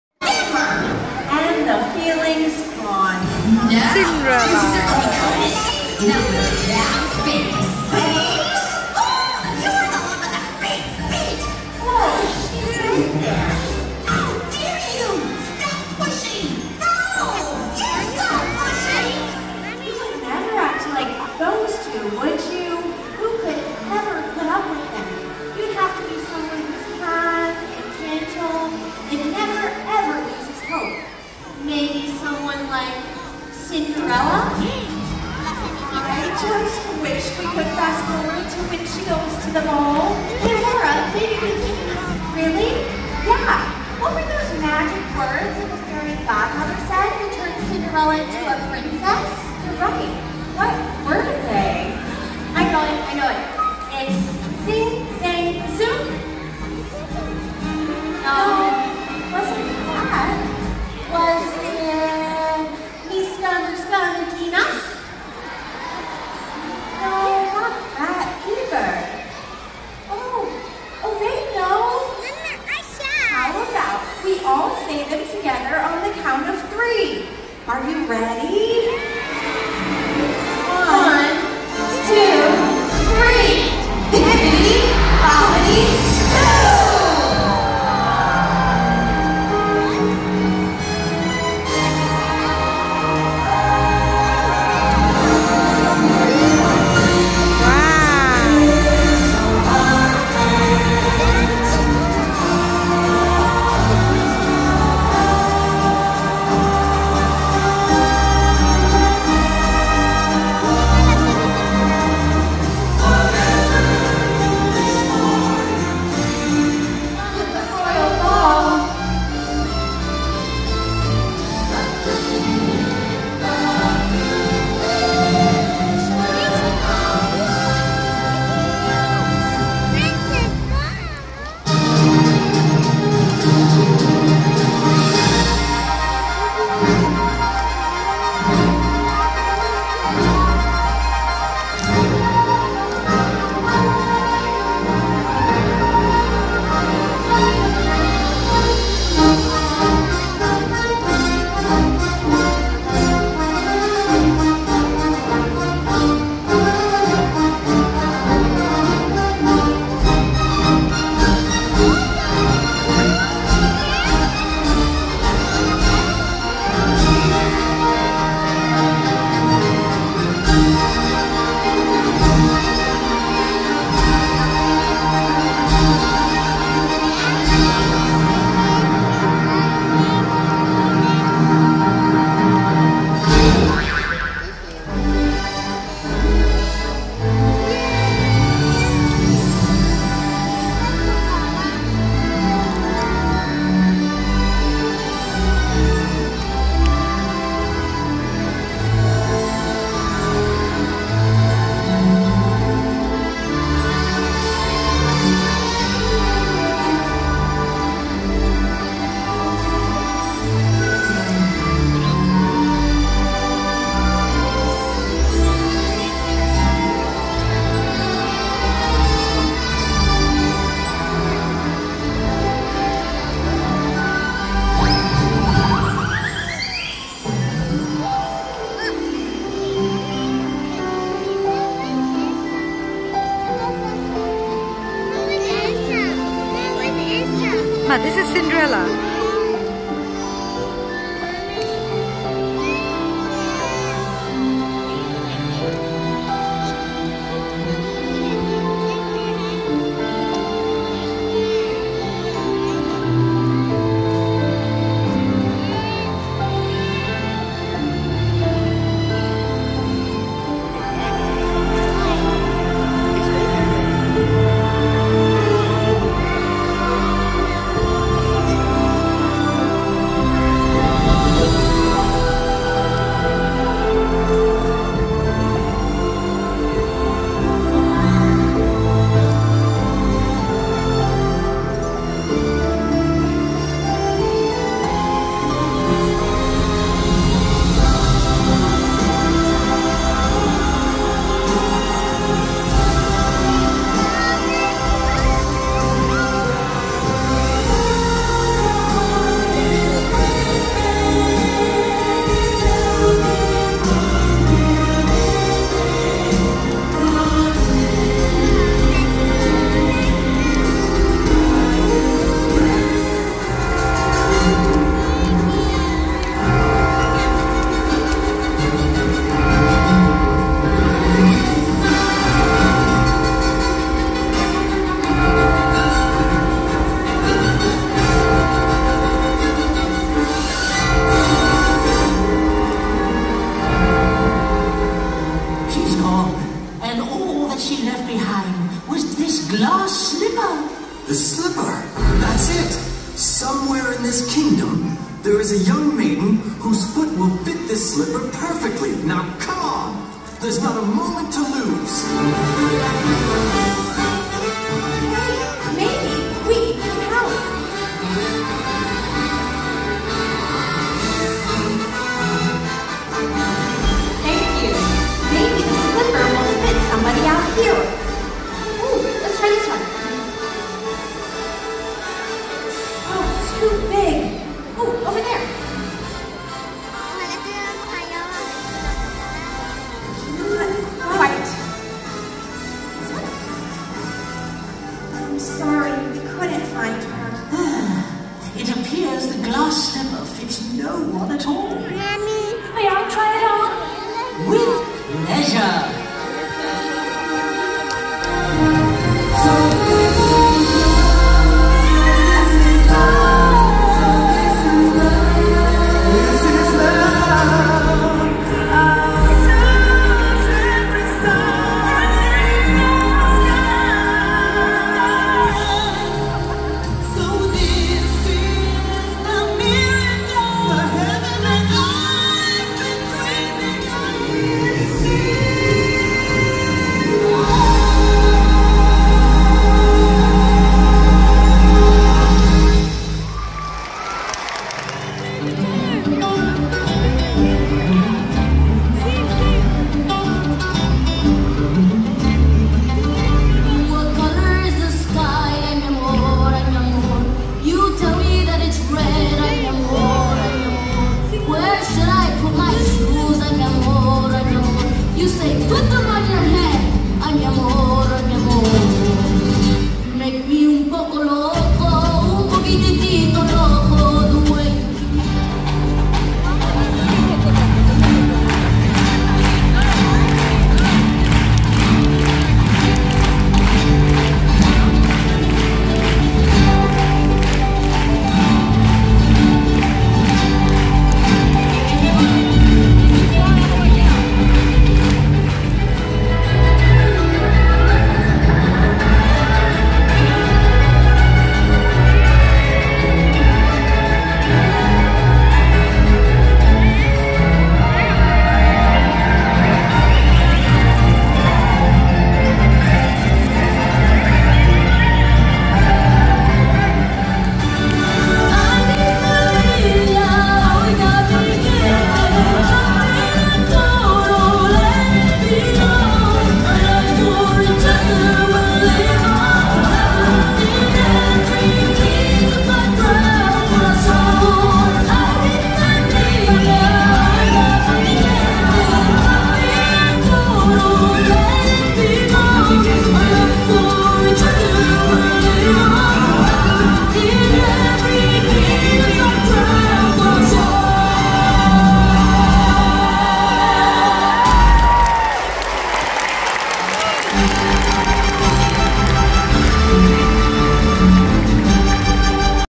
Disney on ice 2023 in Texas || Disney Princess Cinderella full live show
Disney on ice is a wonderful ice skating show of Disney characters. kids love this show. we really enjoy the show. here is a glimpse of the show of princess Cinderella .